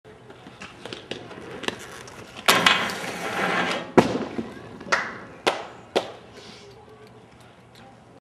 Topsoul handrail.
Me doing a topsoul down a 13 stair rail.